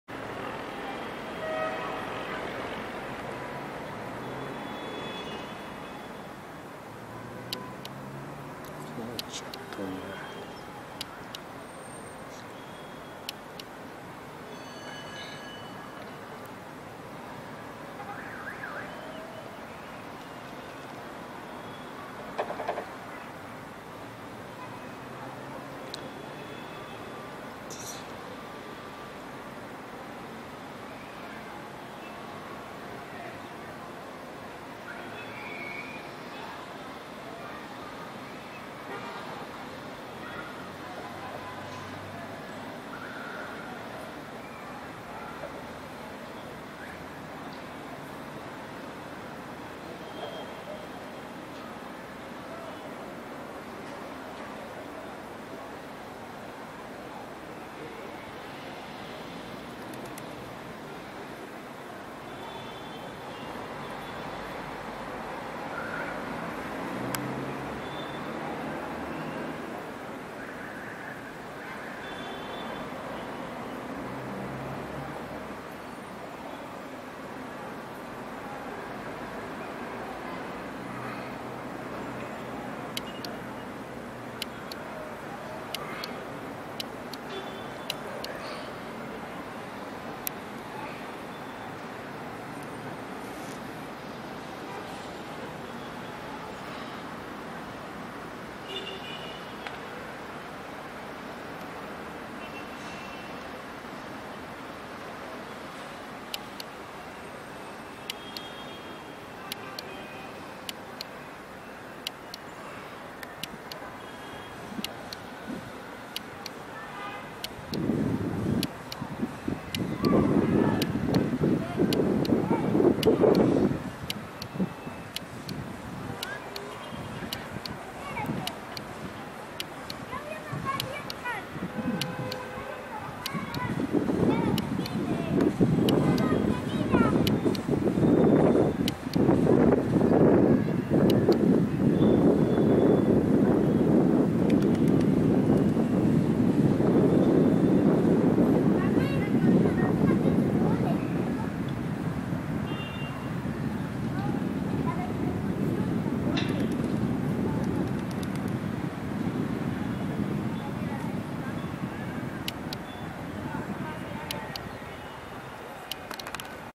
flamante cancha sintética del estadio Héroes de San Ramón
Y AQUÍ JUGARÁ UTC EN LA FLAMANTE CANCHA SINTÉTICA DEL ESTADIO HÉROES DE SAN RAMÓN QUE SE INAUGURA ESTE VIERNES Y SERÁ CON EL PARTIDO UTC VS REAL GARCILASO POR LA FECHA 15 DE LA LIGA 1 MOVISTAR.